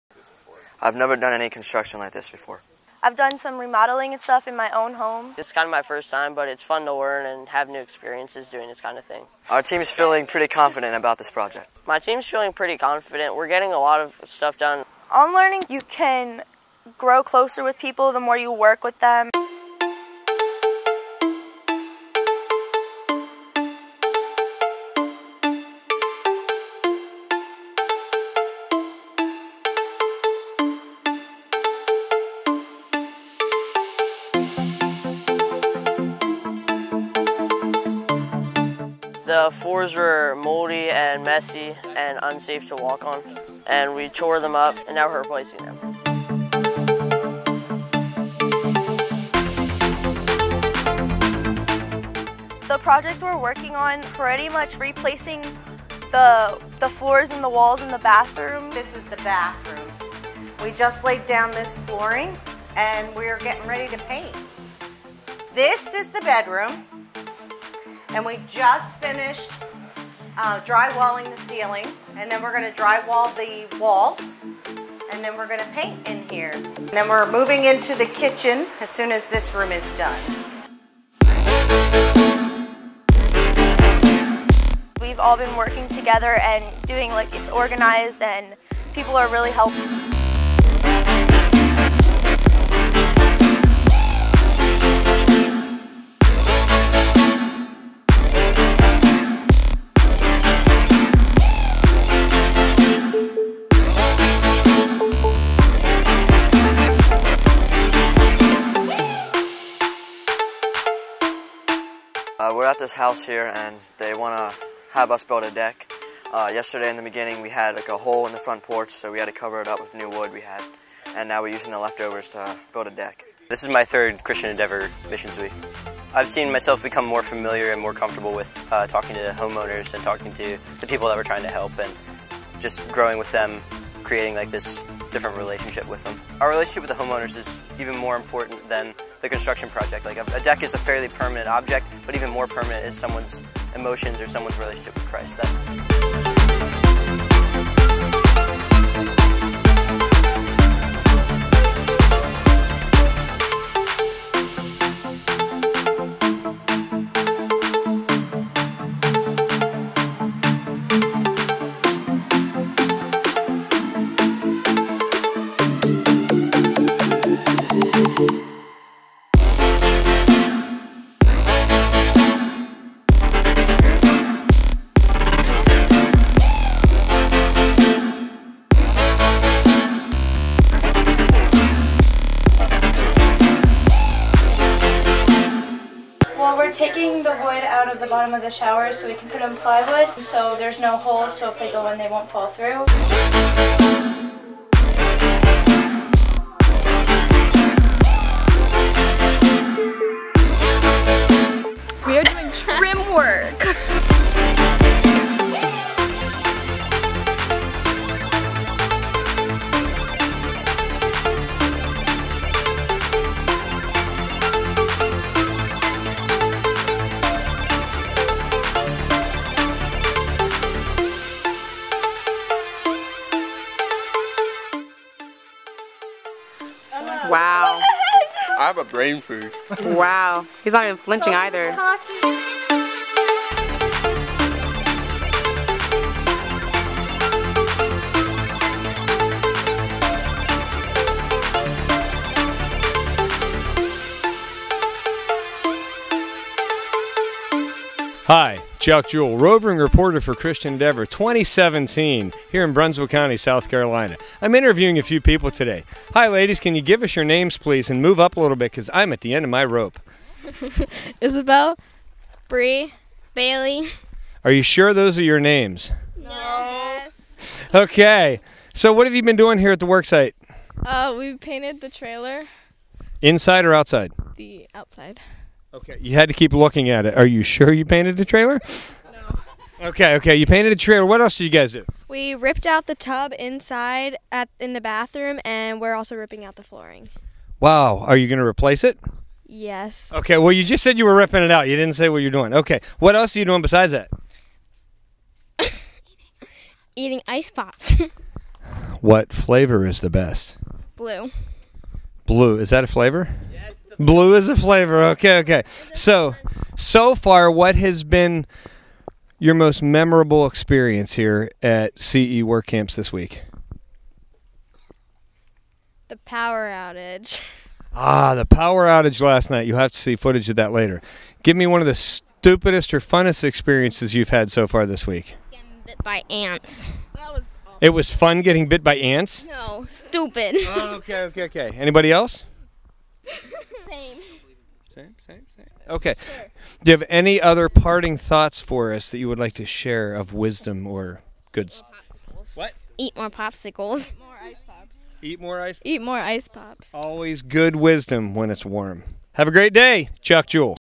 CE Missions Week 2017 | Tuesday, Week 3
Tuesday of Week 3 was a great day of progress. The teams did a lot of Painting, Deck Construction, Floor Repair and Bathroom replacement.